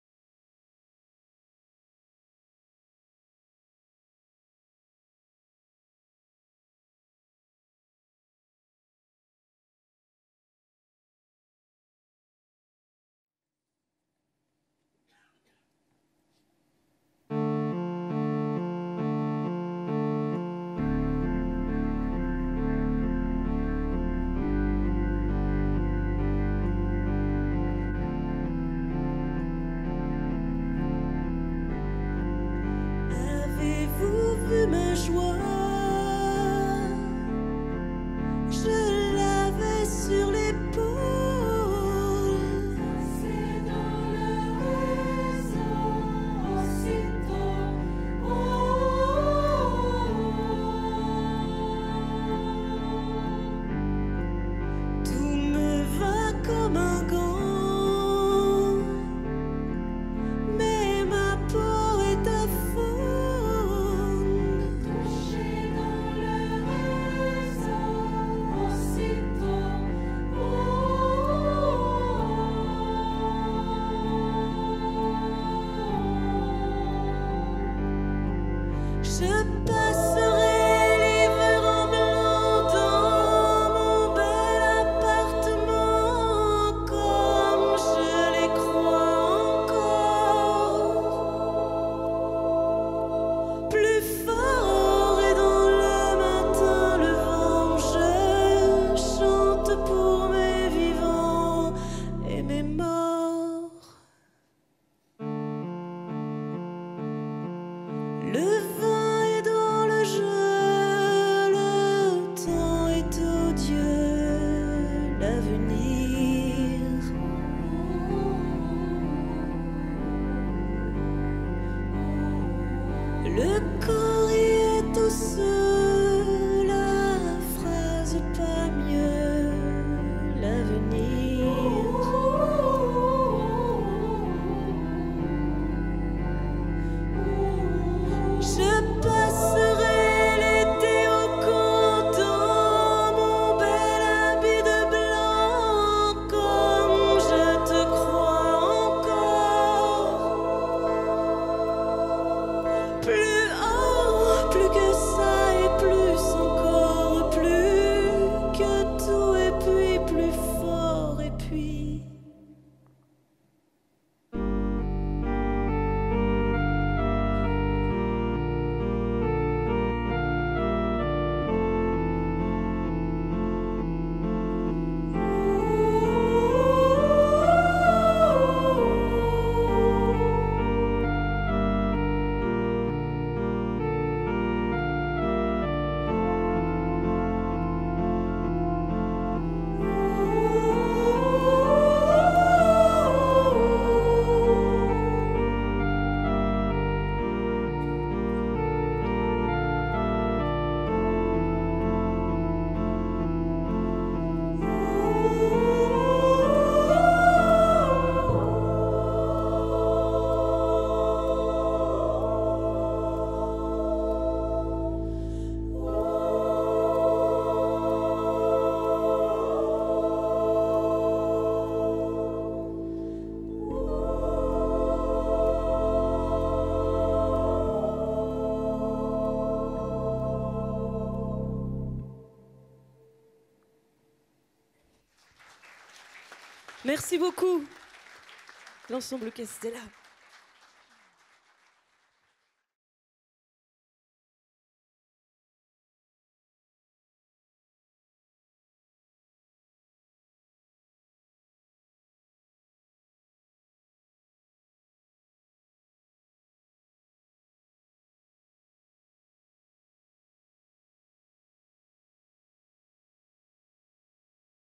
Katel et l'Ensemble Vocal Castella à La Biscuiterie 2
♫ La Biscuiterie ♫

Prestation de Katel et de l'Ensemble Vocal Castella à La Biscuiterie de Château-Thierry.